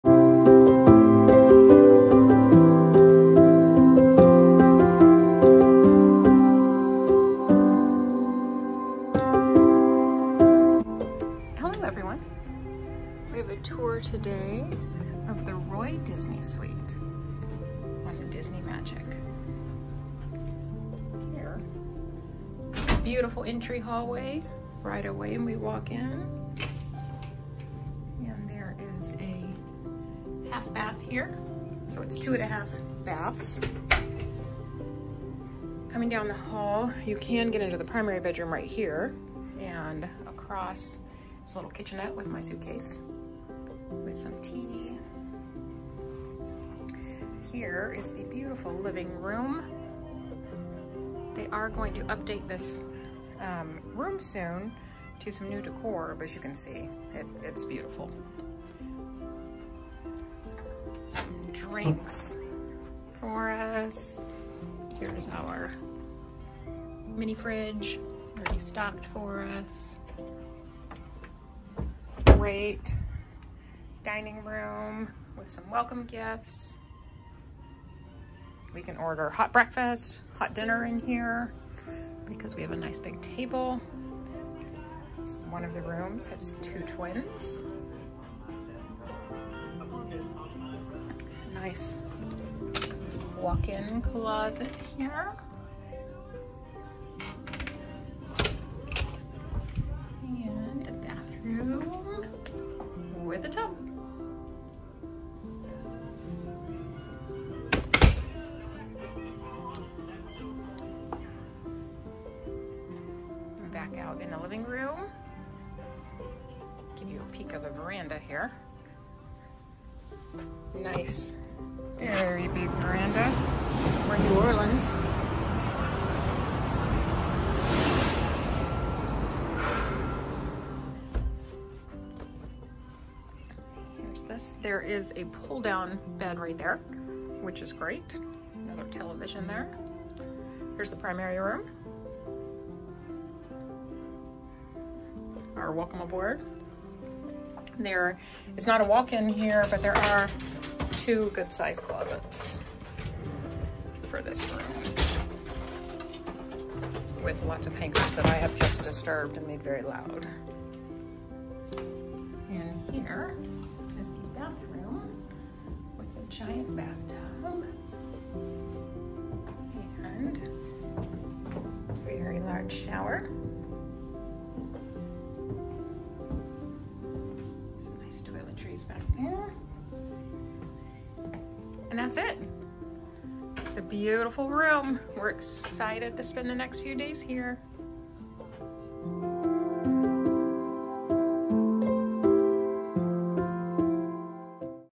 Roy O. Disney Suite on the Disney Magic Disney Cruise Line
Hello and welcome! In today’s video, we have a tour of the Roy O. Disney Suite on the Disney Magic- Disney Cruise Line!

We would love to help you plan your next Disney vacation! Find us at Storybookdestinations.com!